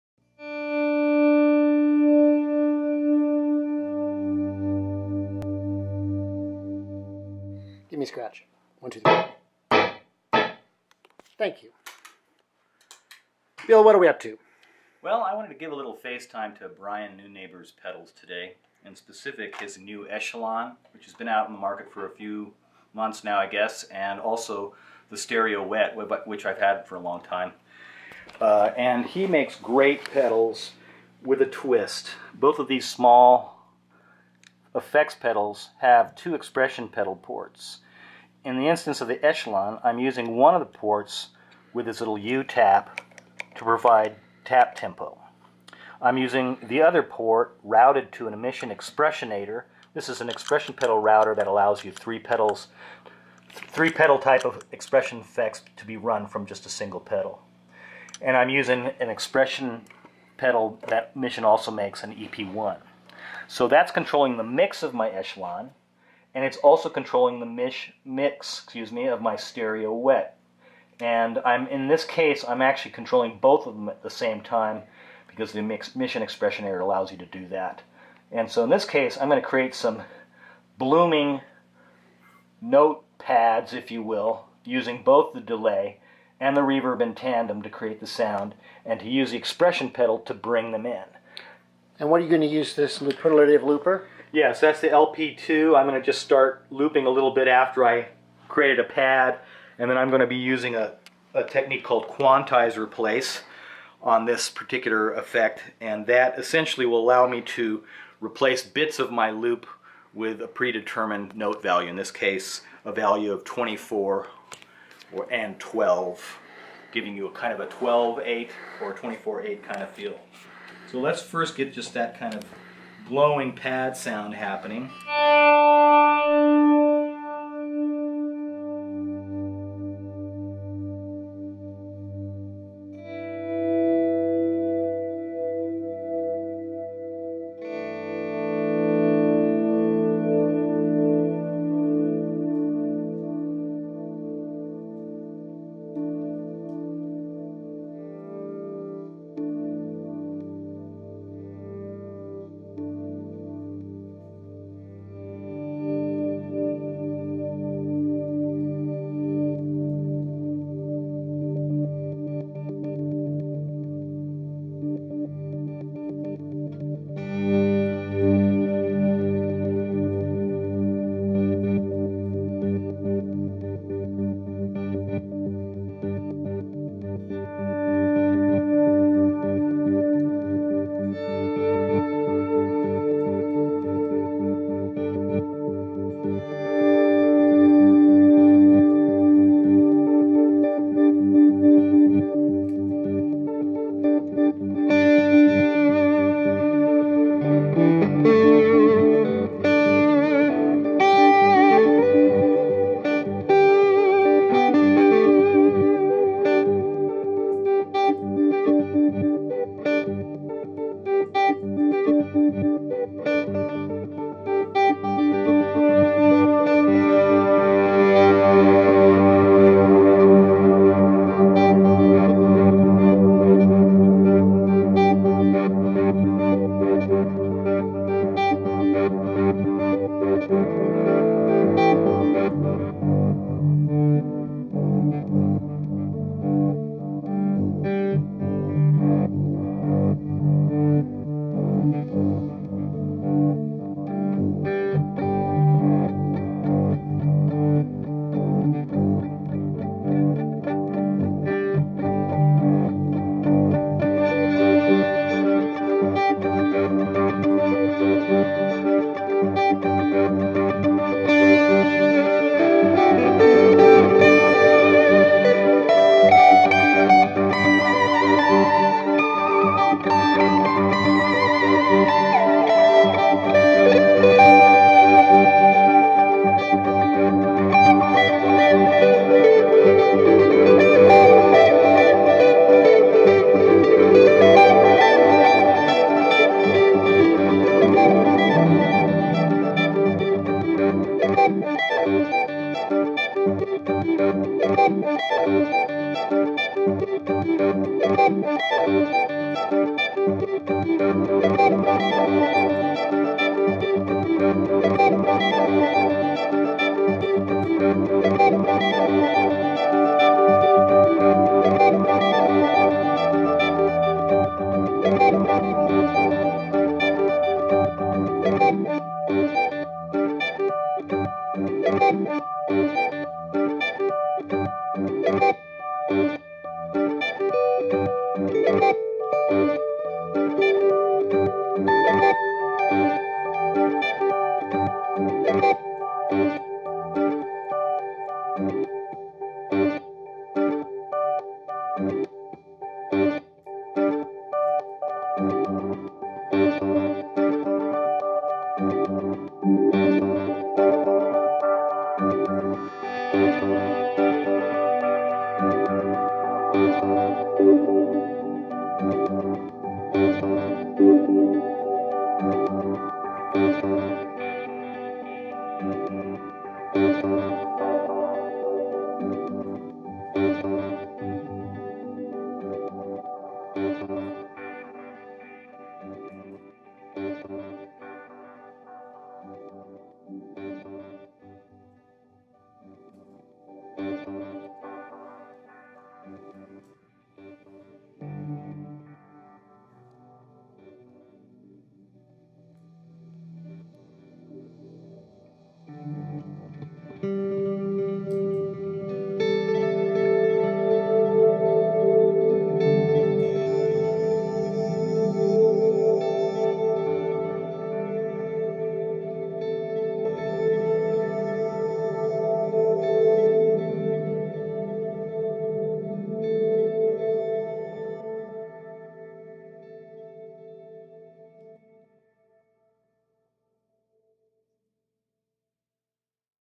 Neunaber Effects Demo with Bill Walker
In this demo I'll be doing some live loop improvisation and real time effects control. . I'll be placing two Neunaber effects before my Looper, a looperlative LP-2, and a Line 6 M-9 afterwards for additional post looper effects and a bit of looping towards the end. I'm using a Mission Expressionator and Ep-1 pedal to control the mix of the two Neunaber effects pedals, an Echelon echo, and a Wet stereo reverb, as well as parameters on the M-9.  The Neunaber pedals are unique among small footprint DSP effects in that they each have two expression pedal ports to control the mix and depth of the effects, as well as customizer software that allows the user to download different versions of the effect, change the pedals routing configuration and swap knob functions, They also sound spectacular, and the expression pedal control allows for the kind of control that one expects in a large footprint reverb or delay but rarely finds in a small pedal format . Im using the following gear in order of signal path : Xotic SP compressor, Hermida Zendrive, Neunaber Echelon stereo echo, Neunaber Wet stereo reverb, Xotic X blender. Looperlative LP-2, Line 6 M-9 Magic Amps AC15 clone, Headstrong Lil King reverb